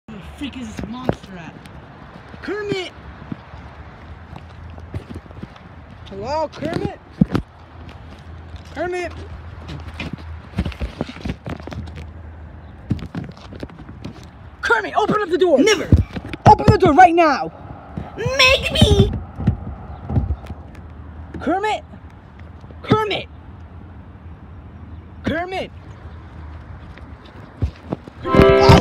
Kermit the Frog 🐸 takes down Erik with a Lexus! (Episode 2)
Kermit has taken Erik to Africa to make him suffer for what he did!